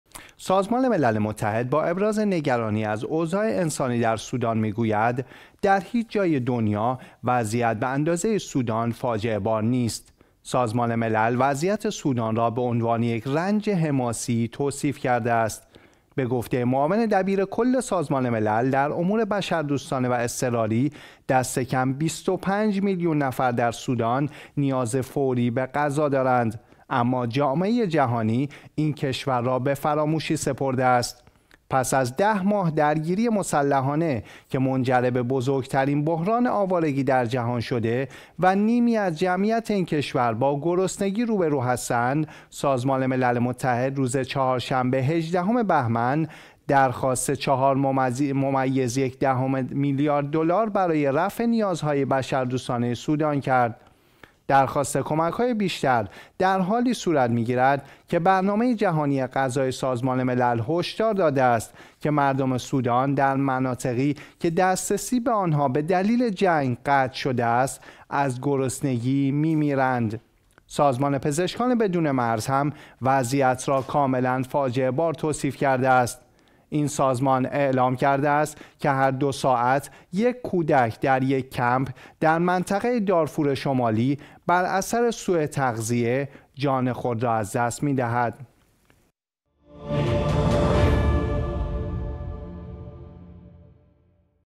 سازمان ملل: در هیچ جای دنیا وضعیت به اندازه سودان فاجعه ‌بار نیست
سازمان ملل متحد با ابراز نگرانی از اوضاع انسانی در سودان می‌گوید: در هیچ جای دنیا وضعیت به اندازه سودان فاجعه‌بار نیست. سازمان ملل وضعیت سودان را به عنوان یک "رنج حماسی" توصیف کرده است.
به گفته ی معاون دبیرکل سازمان ملل در امور بشردوستانه و اضطراری، دستکم ۲۵ میلیون نفر در سودان نیاز فوری به غذا دارند اما جامعه جهانی این کشور را به فراموشی سپرده است.
پس از ده ماه درگیری مسلحانه که منجر به بزرگترین بحران آوارگی در جهان شده و نیمی از جمعیت این کشور با گرسنگی روبرو هستند، سازمان ملل متحد روز چهارشنبه 18 بهمن، درخواست 4.1 میلیارد دلار برای رفع نیازهای بشردوستانه سودان کرد.
درخواست کمک های بیشتر در حالی صورت می گیرد که برنامه جهانی غذای سازمان ملل هشدار داده است که مردم سودان در مناطقی که دسترسی به آنها به دلیل جنگ، قطع شده است از گرسنگی می میرند.
سازمان پزشکان بدون مرز هم وضعیت را "کاملاً فاجعه بار" توصیف کرده است. این سازمان اعلام کرده است که هر دو ساعت یک کودک در یک کمپ در منطقه دارفور شمالی بر اثر سوء تغذیه جان خود را از دست می دهد.